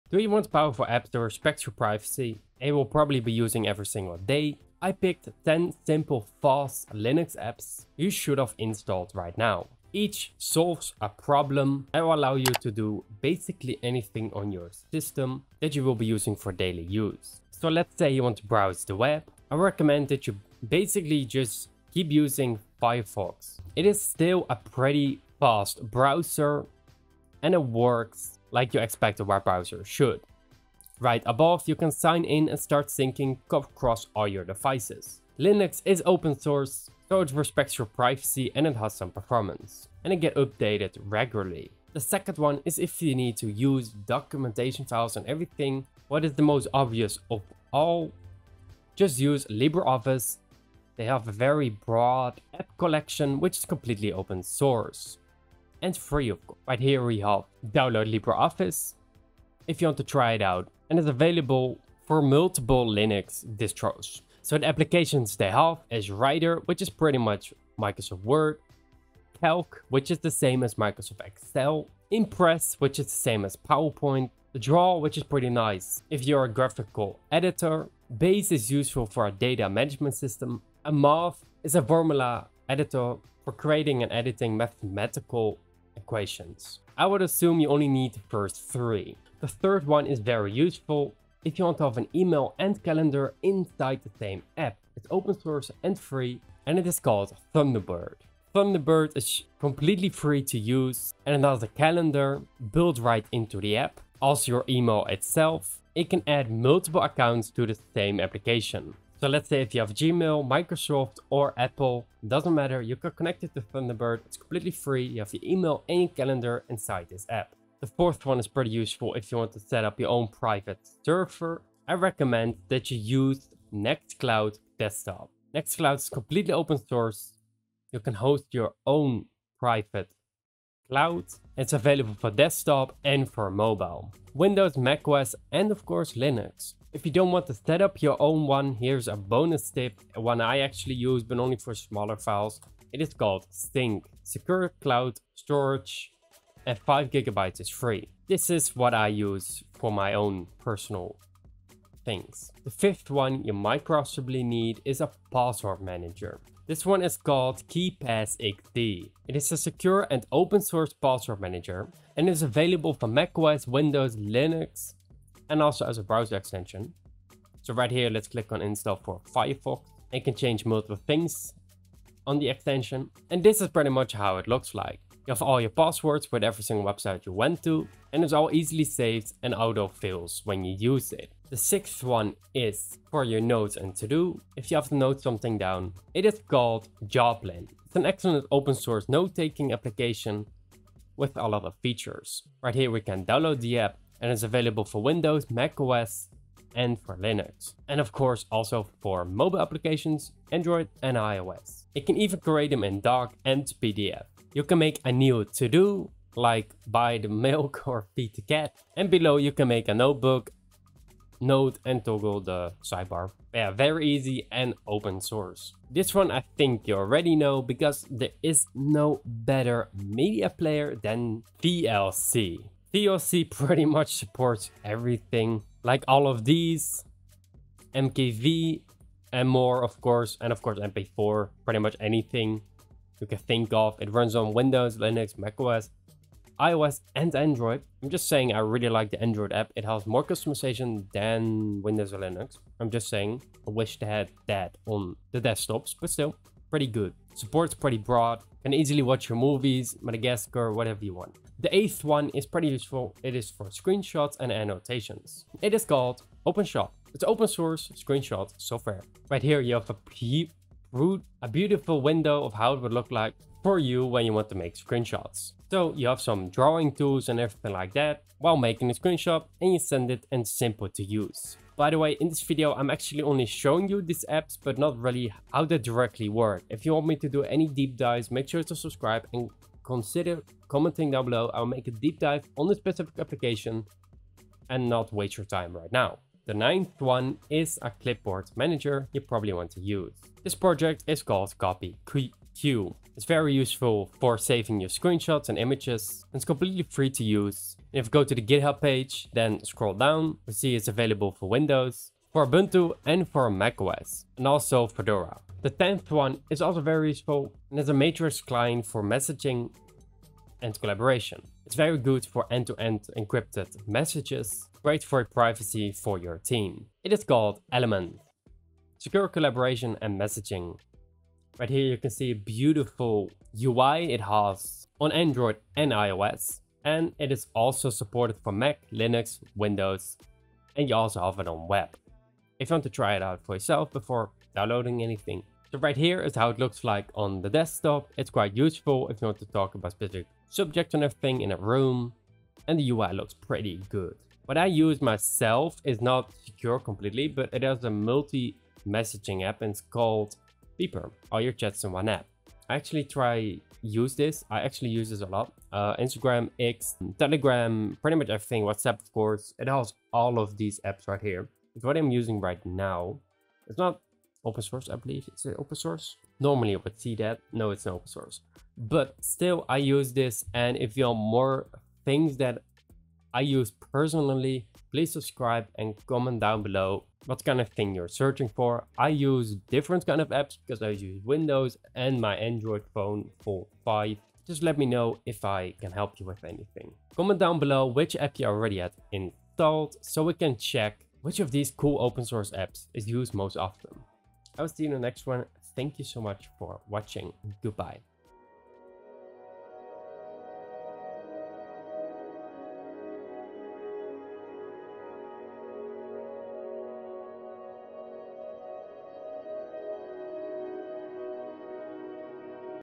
10 Essential Free FOSS Apps Every Linux User Should Have
🔥Summary🔥
10 Best FOSS Linux Apps You'll Actually Use Every Day — Firefox, LibreOffice, Thunderbird, KeePassXC, Syncthing, Flameshot, VLC, GIMP, Kdenlive and OBS Studio give you privacy, productivity, media, and creative power on any distro in 2025, perfect for daily workflows and developers alike.

✨Timestamps✨
0:00 - Hello There!
1:00 - Educational Advice
2:00 - Make Sure To Subscribe Too!